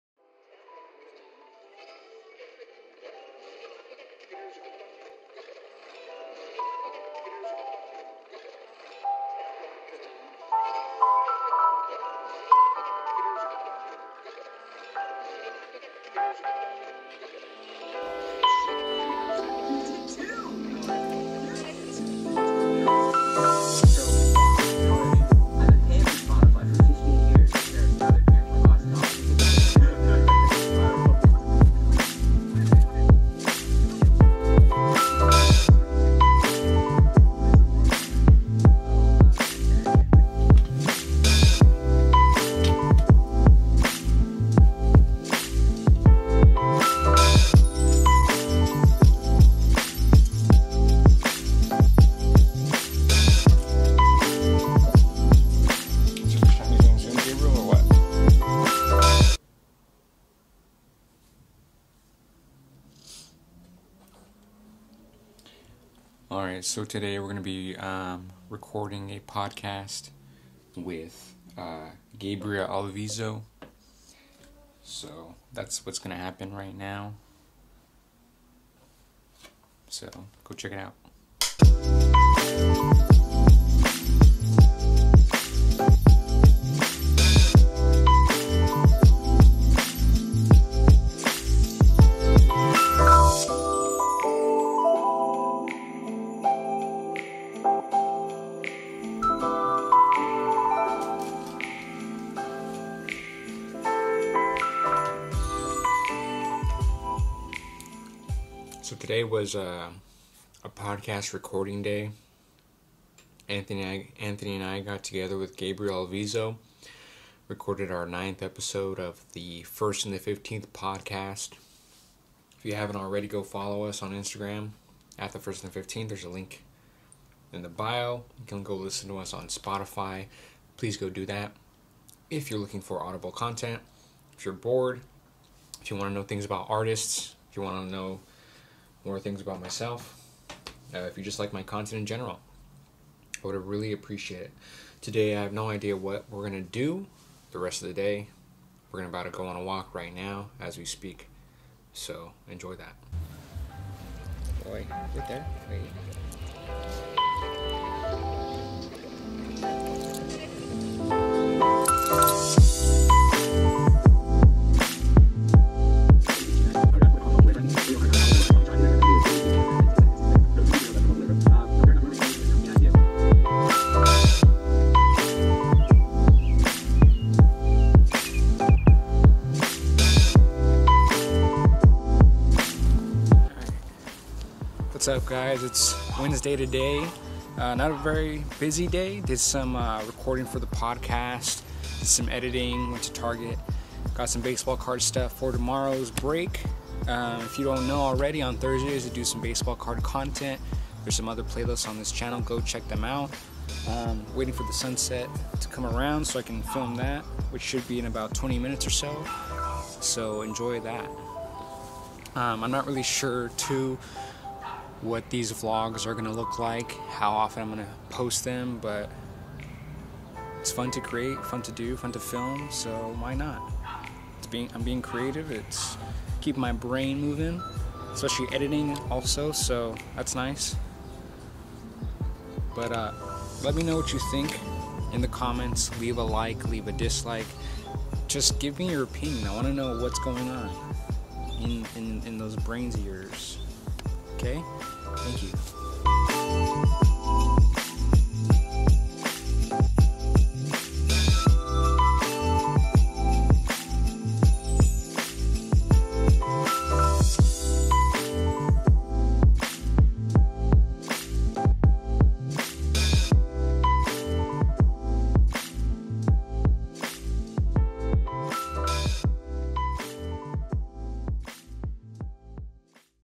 I'm a Podcaster
Today, Anthony and I recorded another episode for our podcast. Django and I went for a walk and it was a chill work day. Follow on all social media platforms.